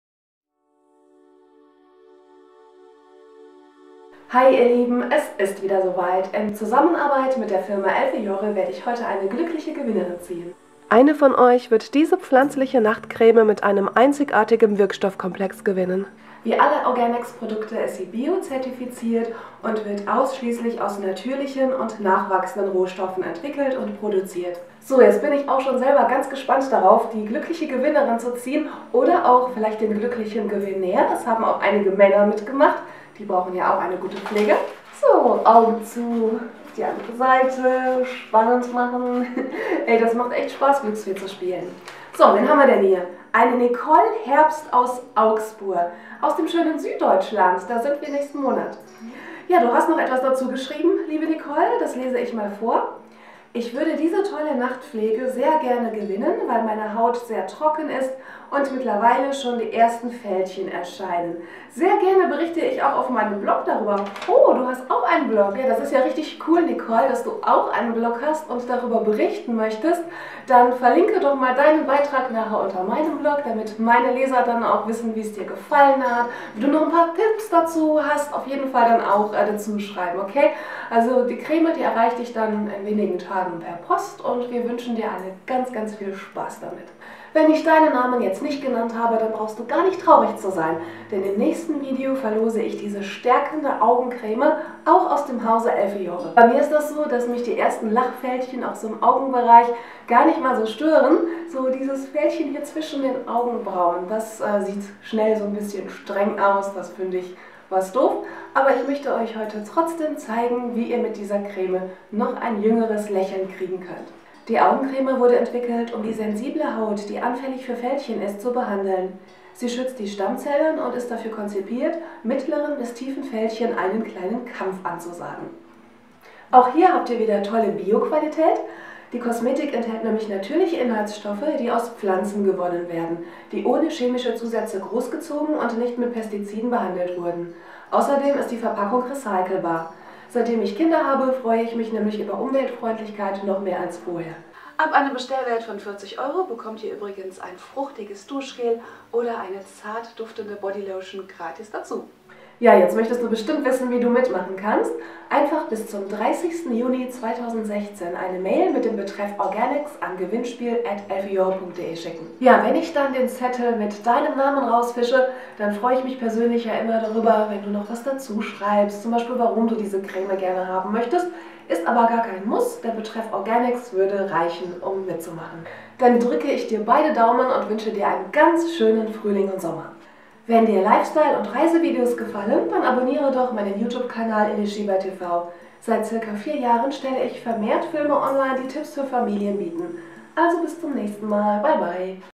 #243 - neue Verlosung und Gewinnerziehung - Biokosmetik von Ellfiore
(Werbung - bezahlte Kooperation) Heute ziehe ich „live“ die Gewinnerin meiner letzten „Biokosmetik Verlosung“ in Zusammenarbeit mit der Firma Ellfiore.

Außerdem könnt ihr bei einem neuen Gewinnspiel mitmachen: Ihr könnt die stärkende Augencreme bekommen. Sie wurde entwickelt, um die sensible Haut, die anfällig für Fältchen ist, zu behandeln. Sie schützt die Stammzellen und ist dafür konzipiert mittleren bis tiefen Fältchen einen kleinen Kampf anzusagen.

Auch hier habt ihr wieder tollle Bioqualität.  Die Kosmetik  enthält natürliche Inhaltsstoffe, die aus Pflanzen gewonnen werden, die ohne chemische Zusätze großgezogen und nicht mit Pestiziden behandelt wurden. Außerdem ist die Verpackung recyclebar – seitdem ich Kinder habe freue ich mich über Umweltfreundlichkeit noch mehr als früher.

Wie du beim Gewinnspiel mitmachen kannst? Einfach bis zum 30.

Ganz viel Glück von Elischeba

Herzliche Grüße von

Elischeba